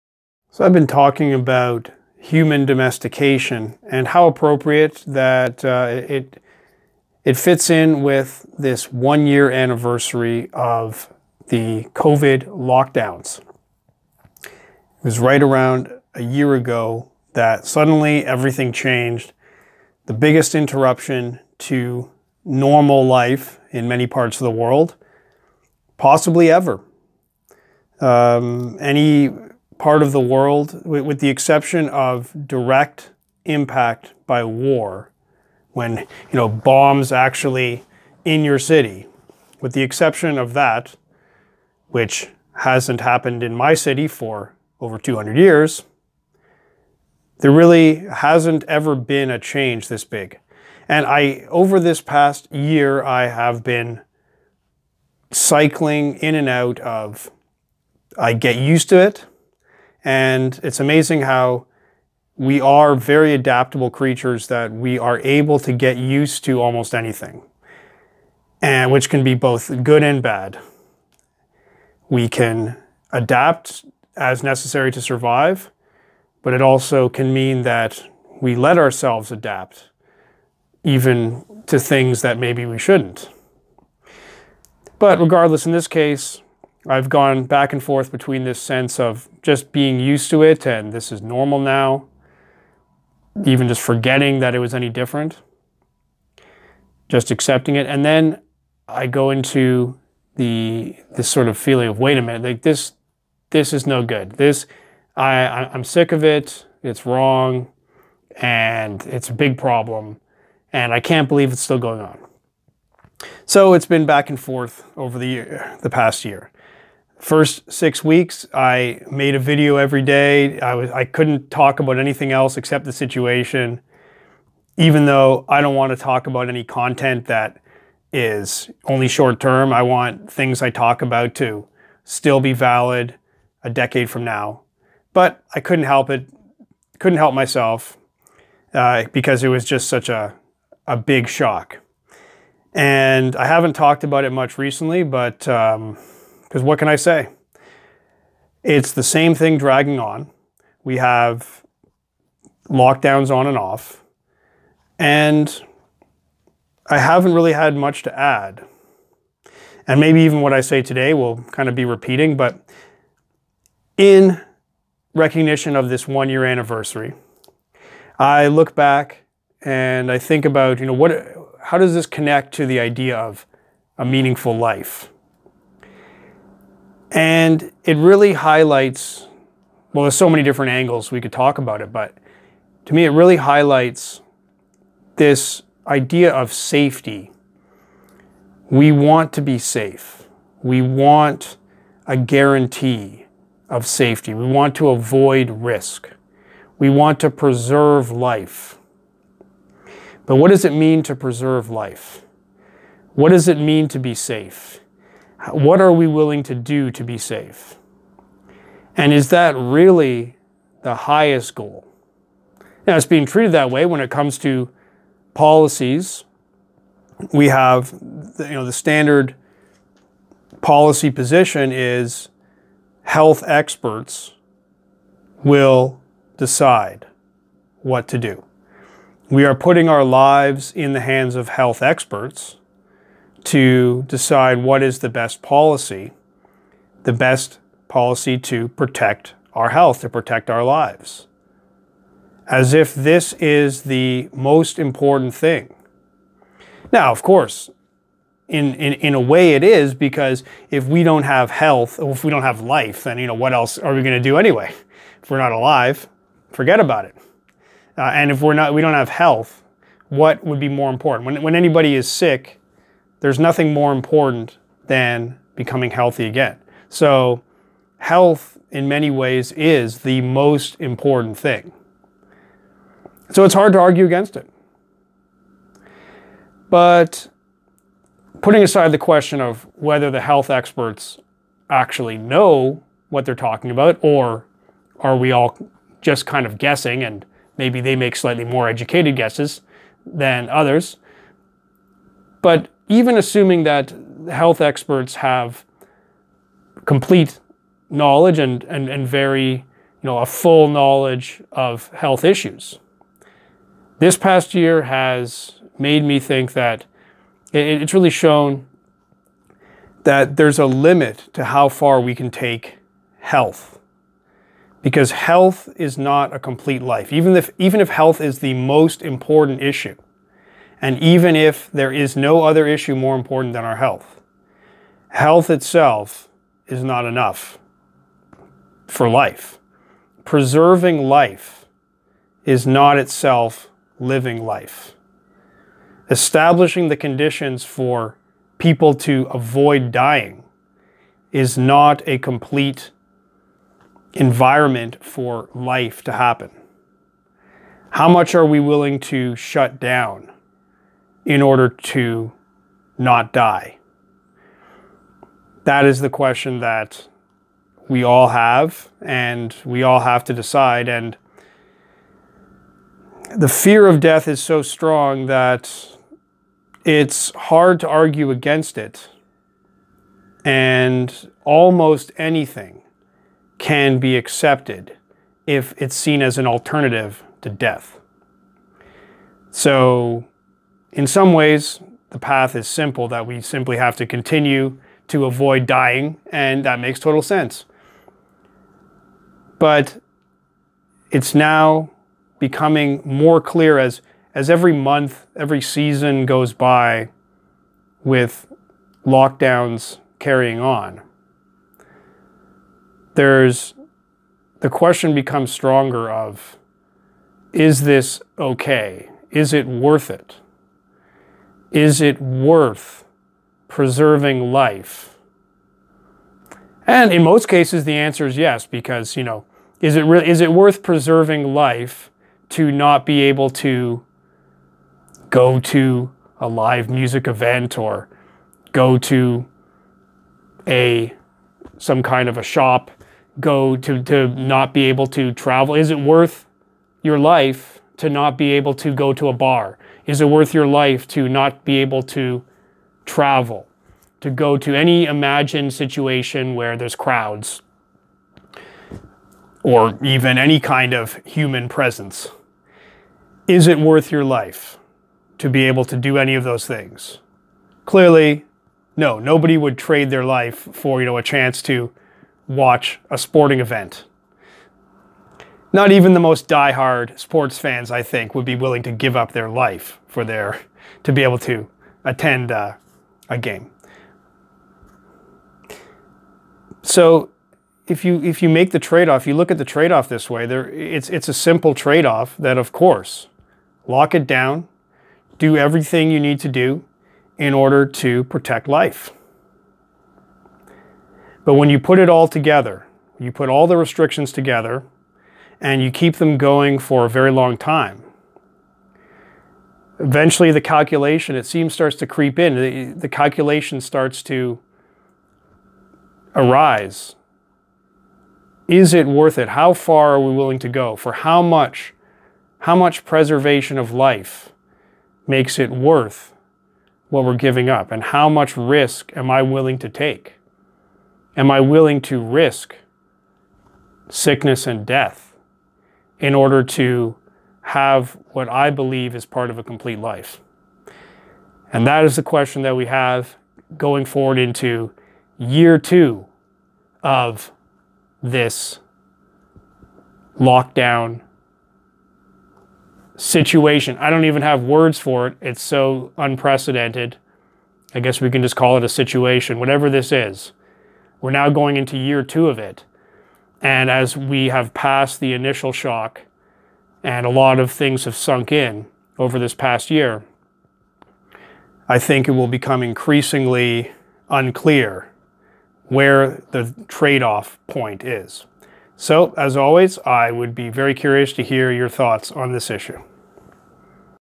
A year of Covid lockdowns: The life and death tradeoff
So I've been talking about human domestication, and how appropriate that it fits in with this one-year anniversary of the Covid lockdowns.
It was right around a year ago that suddenly everything changed, the biggest interruption to normal life, in many parts of the world, possibly ever.
Any part of the world, with the exception of direct impact by war, when bombs actually in your city: with the exception of that - which hasn't happened in my city for over 200 years - there really hasn't ever been a change this big.
And over this past year, I have been cycling in and out of I get used to it.
And it's amazing how we are very adaptable creatures, that we are able to get used to almost anything, which can be both good and bad.
We can adapt as necessary to survive, but it also can mean that we let ourselves adapt even to things that maybe we shouldn't.
But regardless, in this case, I've gone back and forth between this sense of just being used to it, and this is normal now, even just forgetting that it was any different, just accepting it, and then I go into this sort of feeling of "Wait a minute.
This is no good.
I'm sick of it, it's wrong, and it's a big problem, and I can't believe it's still going on."
So it's been back and forth over the past year.
I couldn't talk about anything else except the situation, even though I don't want to talk about any content that is only short-term.
I want things I talk about to still be valid a decade from now.
But I couldn't help it, couldn't help myself, because it was just such a big shock.
And I haven't talked about it much recently, because what can I say?
It's the same thing dragging on.
We have lockdowns on and off, and I haven't really had much to add, and maybe even what I say today will kind of be repeating.
But in recognition of this one-year anniversary, I look back and I think about how does this connect to the idea of a meaningful life?
And it really highlights- well, there's so many different angles we could talk about it, but to me it really highlights this idea of safety.
We want to be safe.
We want a guarantee of safety.
We want to avoid risk.
We want to preserve life.
But what does it mean to preserve life?
What does it mean to be safe?
What are we willing to do to be safe?
And is that really the highest goal?
Now, it's being treated that way when it comes to policies.
We have the standard policy position is "Health experts will decide what to do."
We are putting our lives in the hands of health experts to decide what is the best policy, the best policy to protect our health, to protect our lives, as if this is the most important thing.
Now, of course, in a way it is, because if we don't have health, or if we don't have life, then what else are we going to do anyway?
If we're not alive, forget about it.
And if we don't have health, what would be more important?
When anybody is sick, there's nothing more important than becoming healthy again.
So health, in many ways, is the most important thing.
So it's hard to argue against it.
But putting aside the question of whether the health experts actually know what they're talking about, or are we all just kind of guessing and maybe they make slightly more educated guesses than others- but even assuming that health experts have complete knowledge, and a full knowledge of health issues, this past year has made me think that- it's really that there's a limit to how far we can take health.
Because health is not a complete life.
Even if health is the most important issue, and even if there is no other issue more important than our health, health itself is not enough for life.
Preserving life is not itself living life.
Establishing the conditions for people to avoid dying is not a complete environment for life to happen.
How much are we willing to shut down in order to not die?
That is the question that we all have, and we all have to decide.
And the fear of death is so strong that it's hard to argue against it, and almost anything can be accepted if it's seen as an alternative to death.
So in some ways, the path is simple, that we simply have to continue to avoid dying, and that makes total sense.
But it's now becoming more clear as every month, every season, goes by, with lockdowns carrying on, the question becomes stronger of Is this OK?
Is it worth preserving life?
And in most cases, the answer is yes.
Because is it worth preserving life to not be able to go to a live music event, or go to some kind of a shop, to not be able to travel?
Is it worth your life to not be able to go to a bar? [...]
Clearly, no. [...]
But when you put it all together, you put all the restrictions together, and you keep them going for a very long time, eventually the calculation, it seems, starts to creep in, the calculation starts to arise: is it worth it?